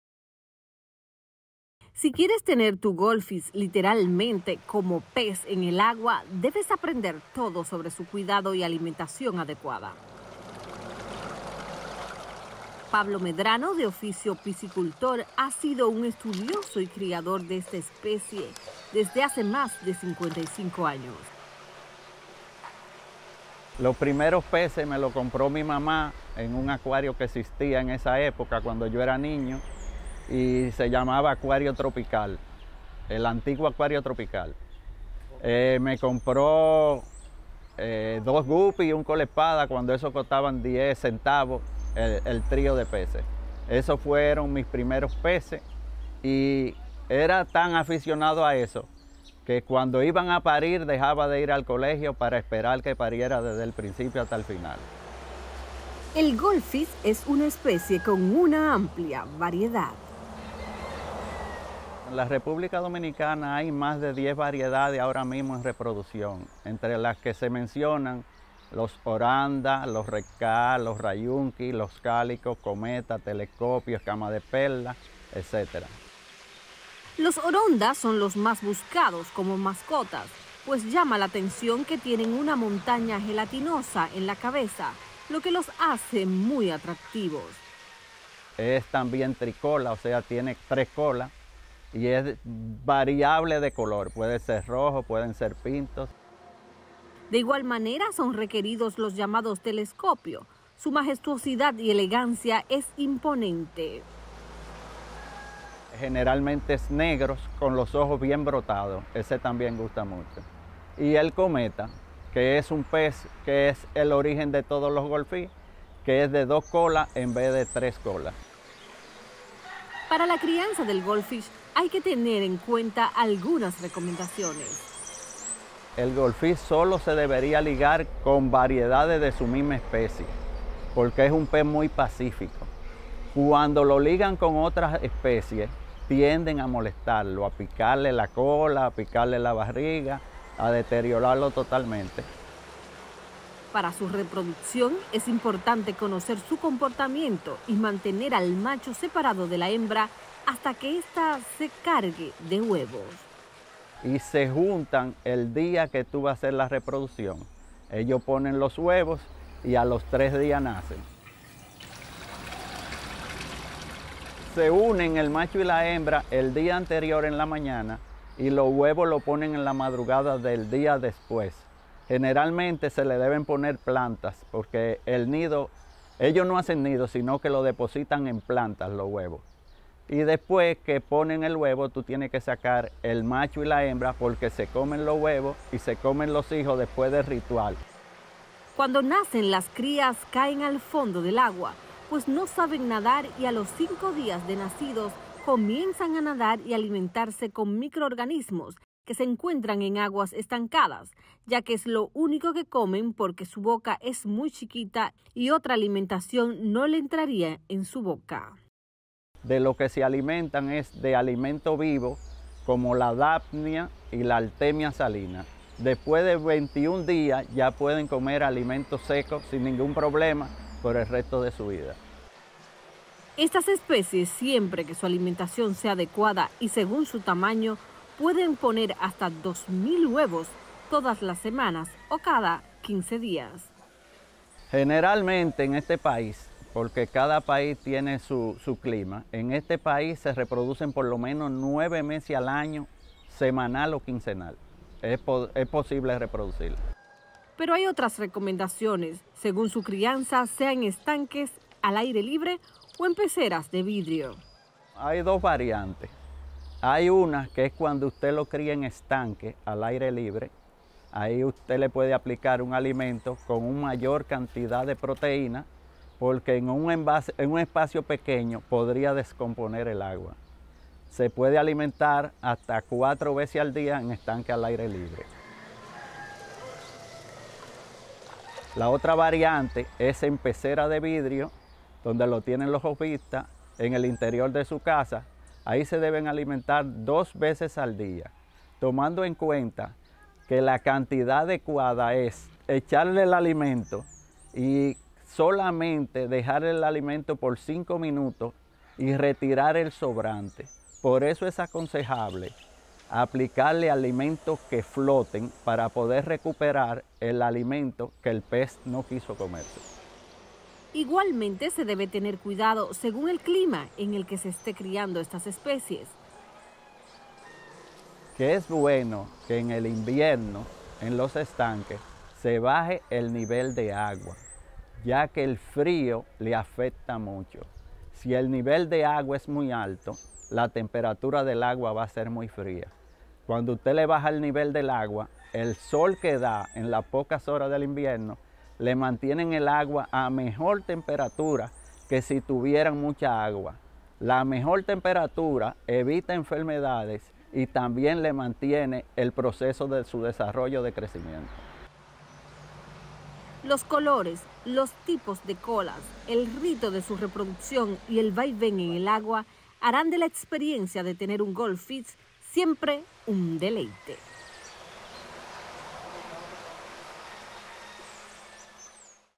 Conozca cómo criar e instalar un criadero de peces Goldfish
Los Goldfish son una de las mascotas más requeridas, tanto por su color como porque son fáciles de cuidar y porque son peces muy tranquilos. Aquí las recomendaciones de Pablito Goldfish con más de 50 años de experiencia en la crianza de estos peces.